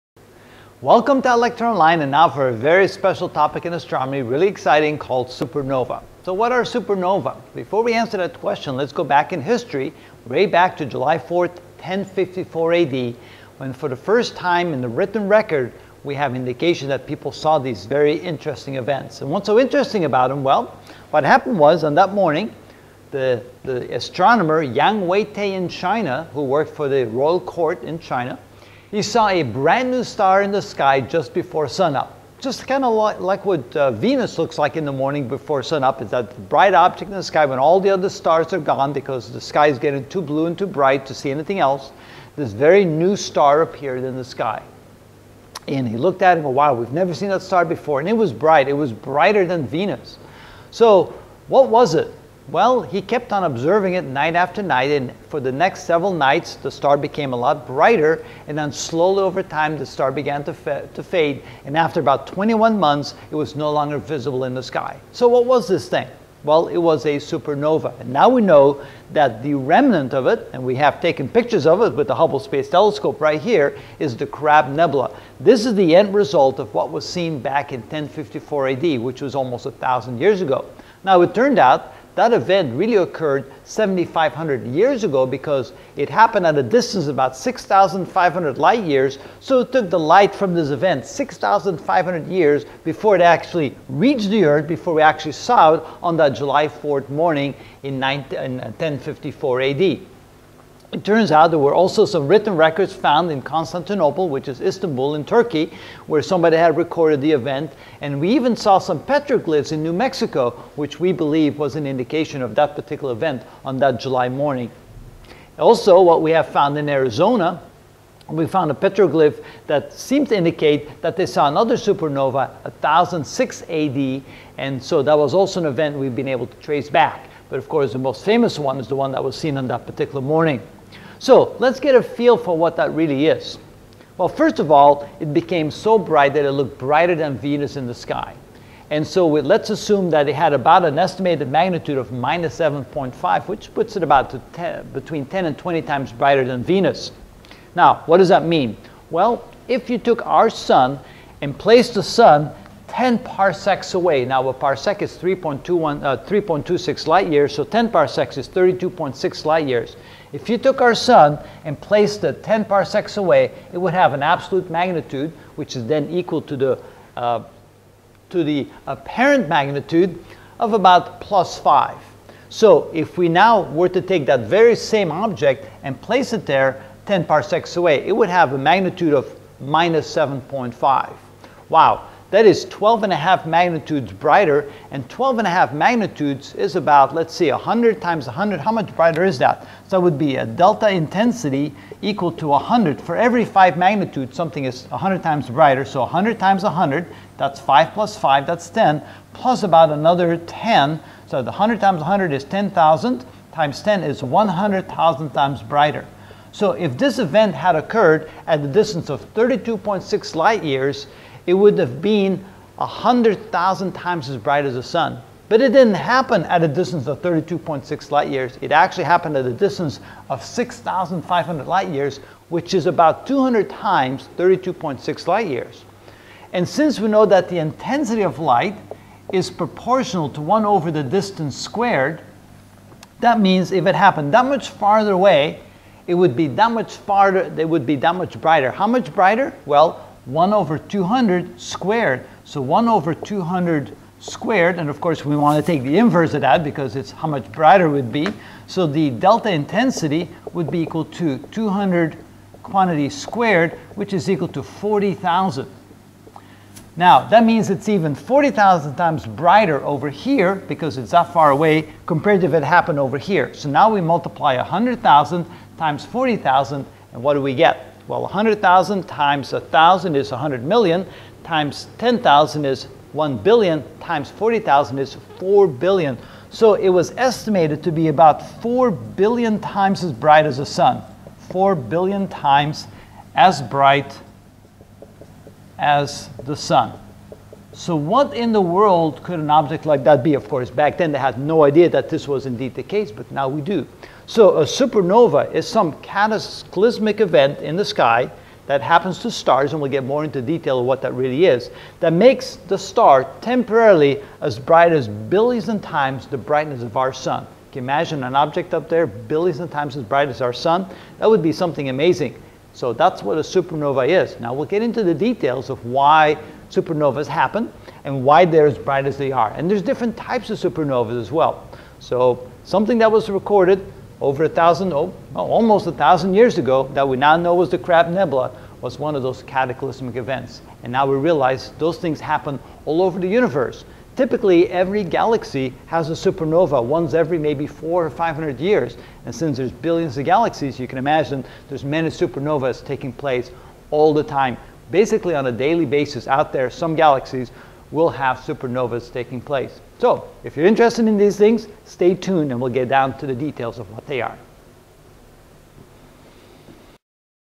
Astronomy: The Supernova (1 of 10) First Observations
In this video I will discuss the Supernova...when and where was it observed, and by whom, etc...